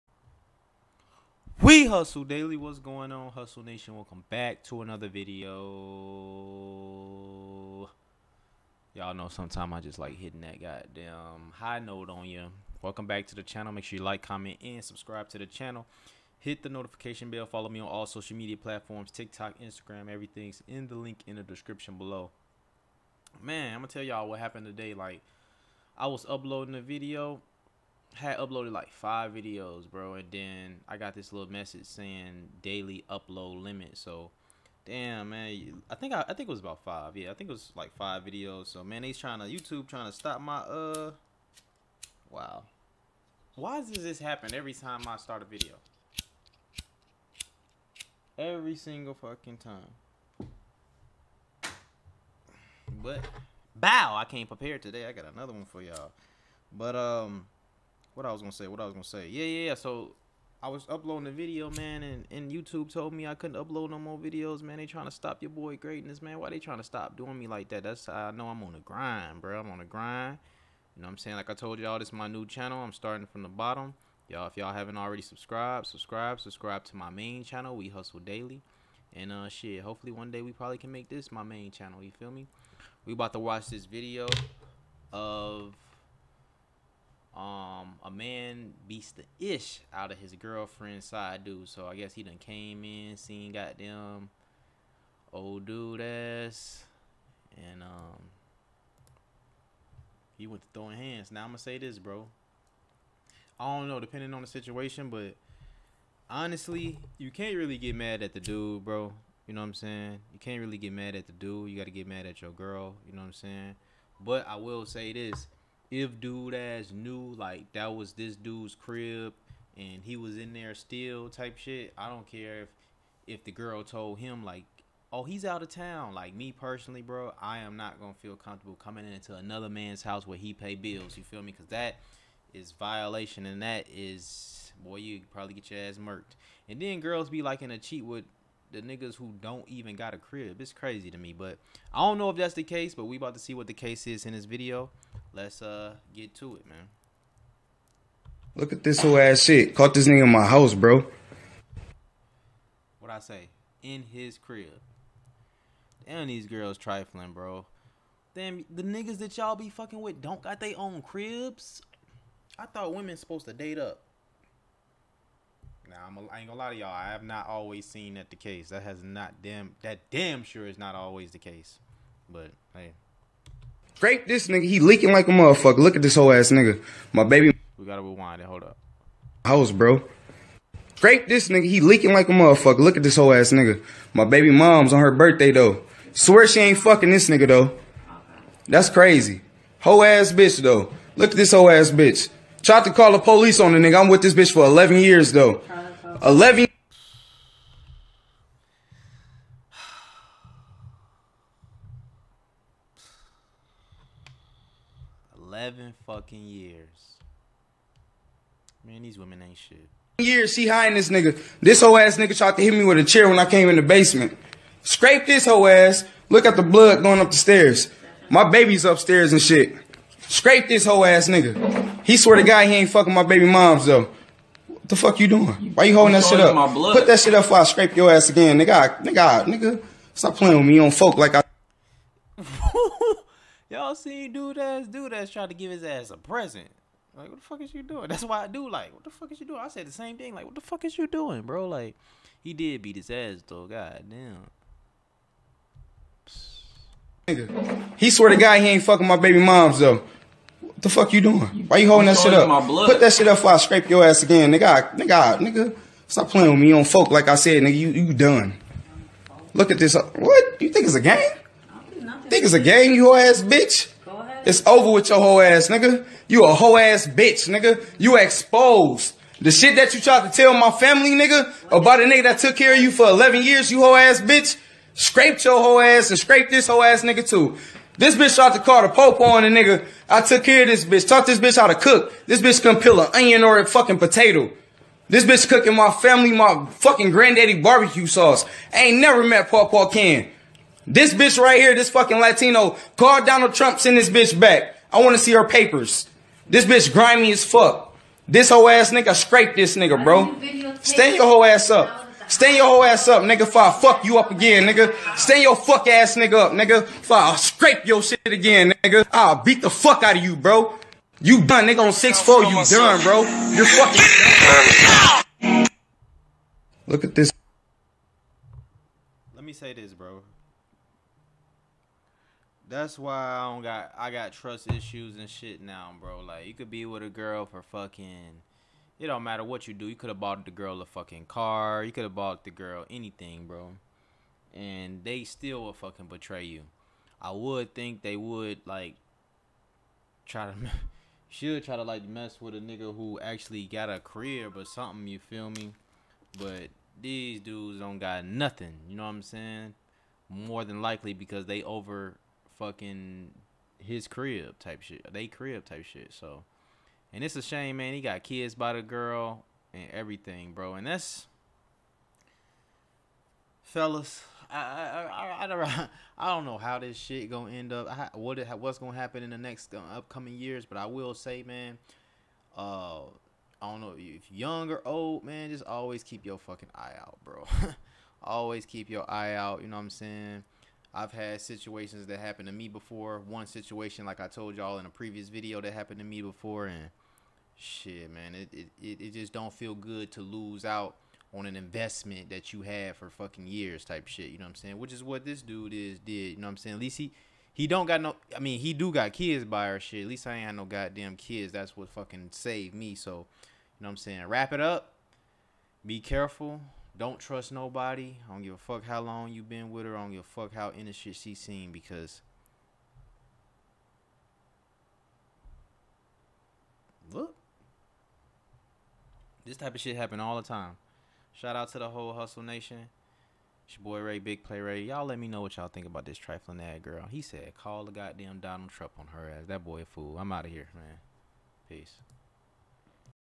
MAN BEAT THE BRAKES OFF HIS GIRLFRIEND SIDE DUDE AFTER CATCHING HIM IN HIS CRIB
Learn How I've Helped Hundreds Of People Buy, Inspect, Flip, & Profit Off Cars ➡️